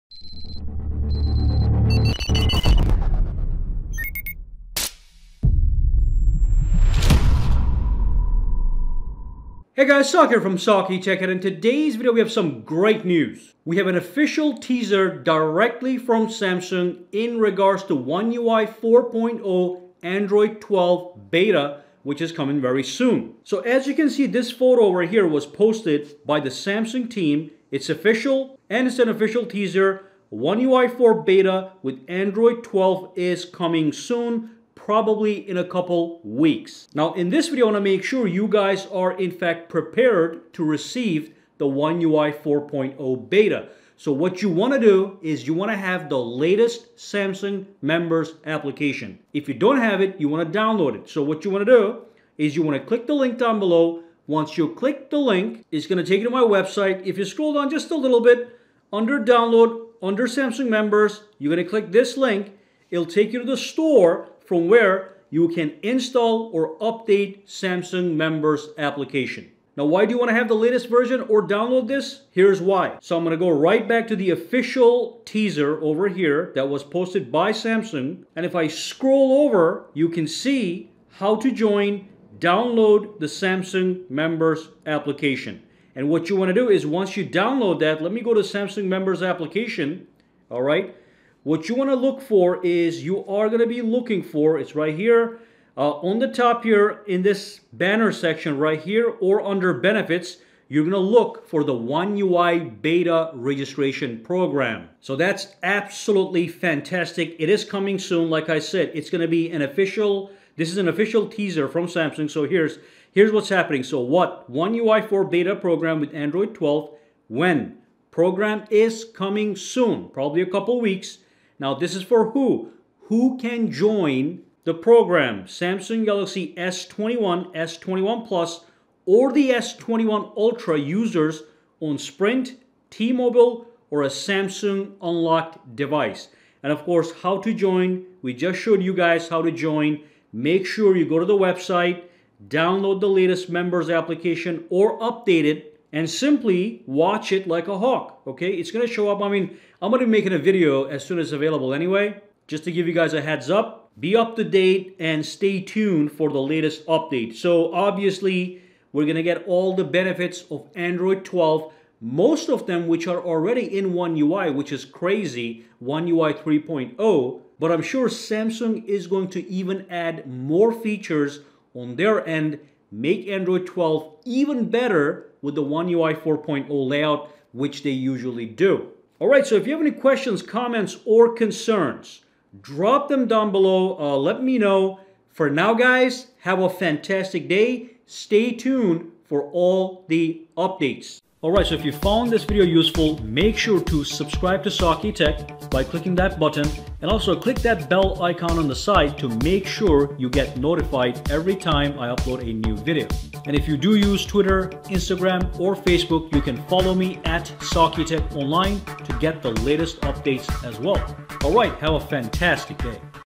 Samsung One UI 4.0 Update with Android 12 - IT'S HAPPENING!
Samsung One UI 4.0 with Android 12 is now official. Samsung team has posted an official teaser of the One UI 4.0 launch on Samsung community.

Let's dive in and see the details. When will we get it and who can sign up for it as well as eligible devices.

Companies can send product to the following address:

Attn: SAKITECH
135 S Springfield Rd. Unit 681